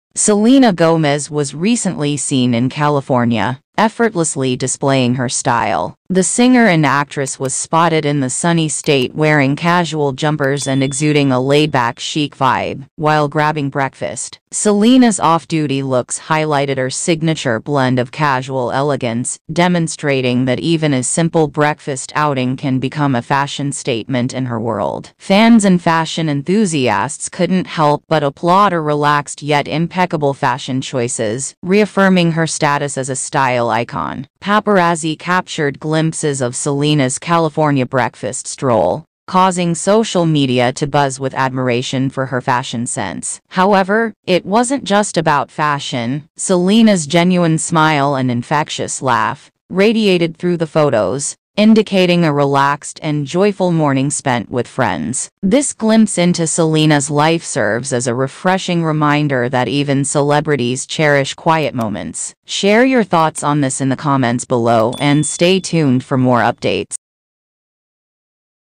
Selena Gomez's Baby Bump looks more VISIBLE than before; evidence of pregnancy ....
selenagomez justinbeiber hollywood selenator selenagomeznews bennyblanco Selena Gomez was recently seen in California, effortlessly displaying her style. The singer and actress was spotted in the sunny state wearing casual jumpers and exuding a laidback chic vibe while grabbing breakfast. Selena's off-duty looks highlighted her signature blend of casual elegance, demonstrating that even a simple breakfast outing can become a fashion statement in her world.

Fans and fashion enthusiasts couldn't help but applaud her relaxed yet impeccable fashion choices,  reaffirming her status as a style icon.  Paparazzi captured glimpses of Selena's California breakfast stroll,  causing social media to buzz with admiration for her fashion sense.  However,  it wasn't just about fashion;  Selena's genuine smile and infectious laugh radiated through the photos,  indicating a relaxed and joyful morning spent with friends.

This glimpse into Selena's life serves as a refreshing reminder that even celebrities cherish quiet moments.